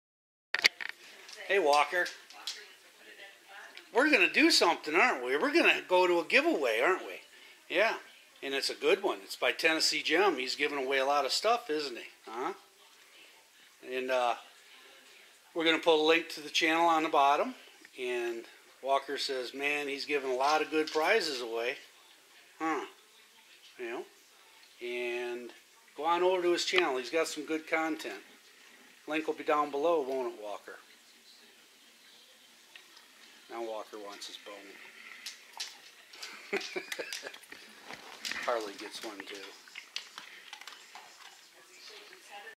Tennessee gem shout out giveaway
Tennessee gem is having a great giveaway . stop in and get in the action as soon as he gets to 1050 giveaway is on. sub stay and watch some good detecting videos.